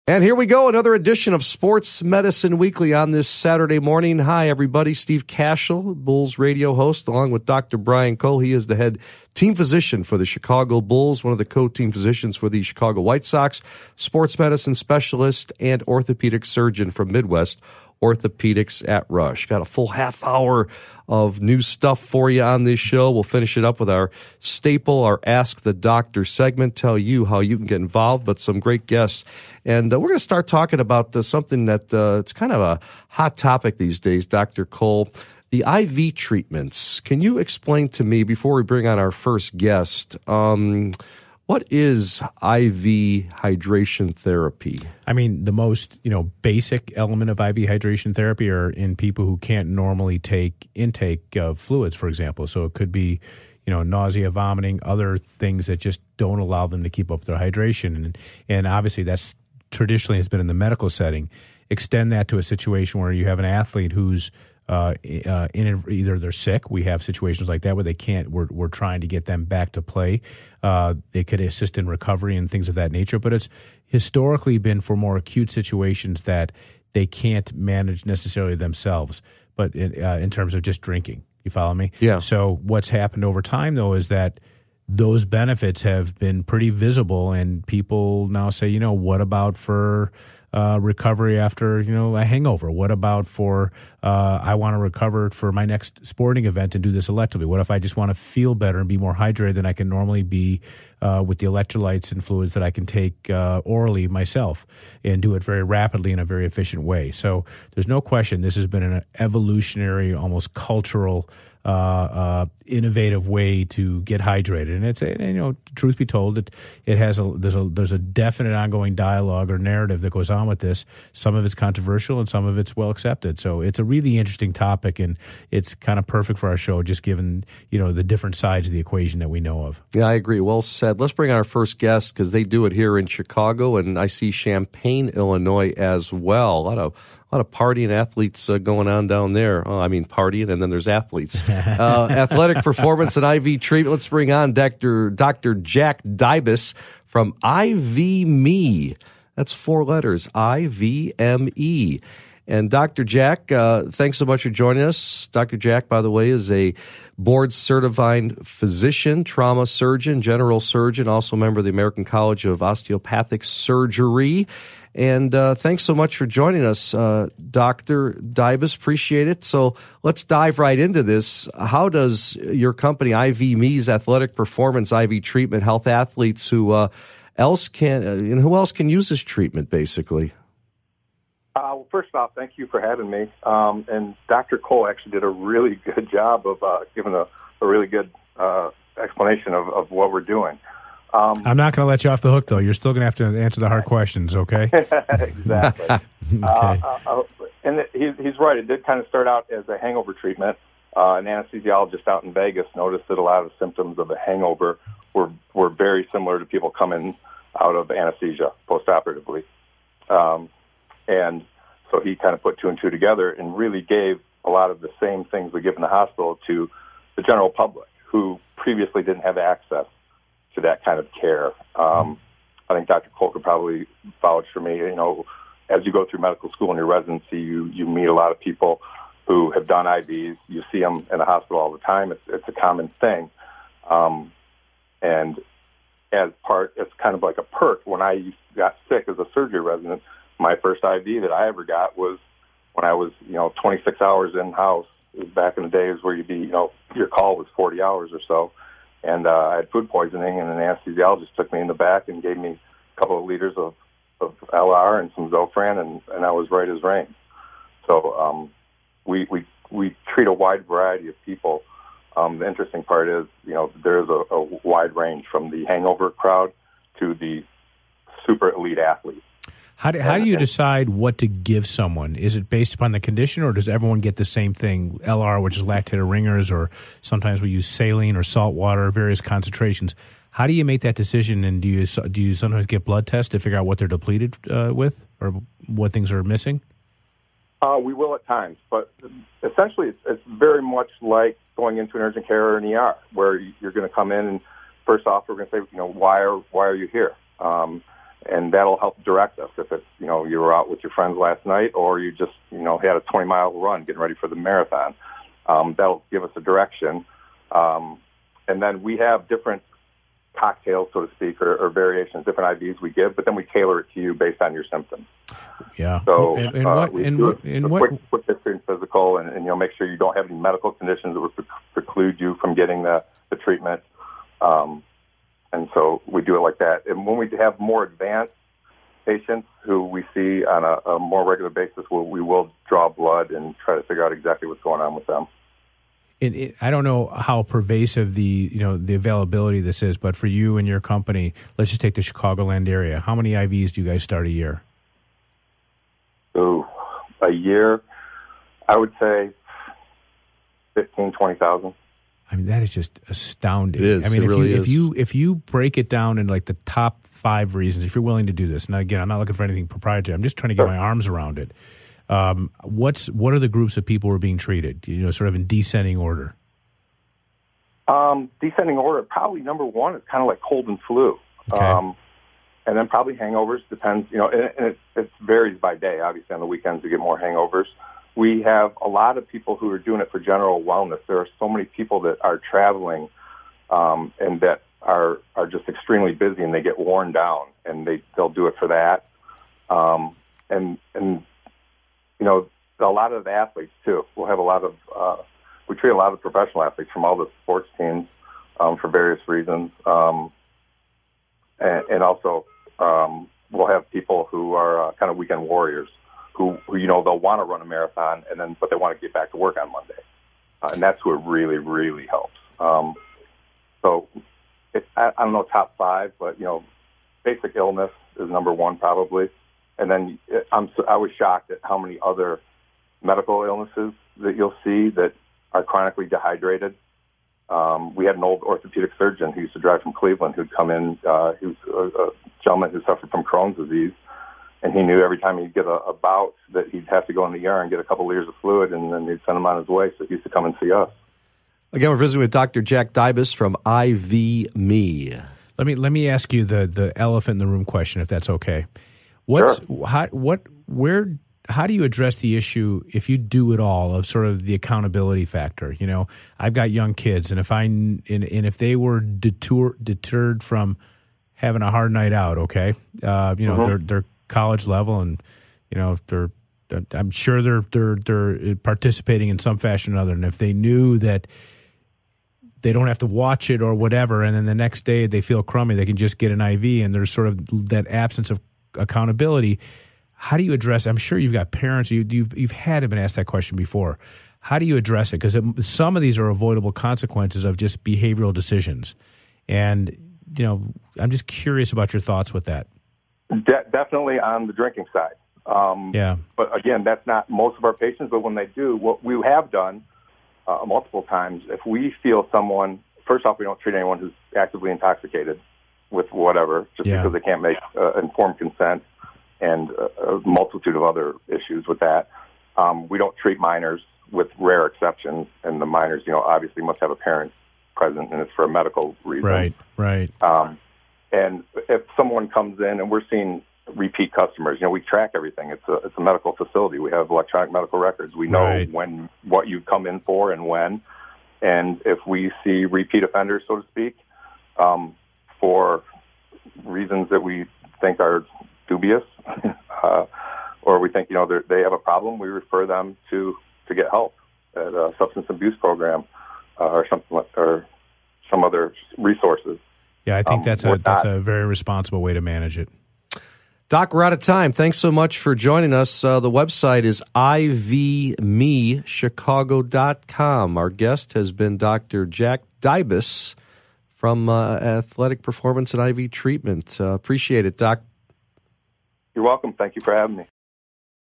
The Evolution of IV Hydration Therapy
Podcast with Dr. Jack Dybis, Dr. Brian Cole and Steve Kashul to discuss the evolving role of IV hydration therapy from traditional hospital use toContinue reading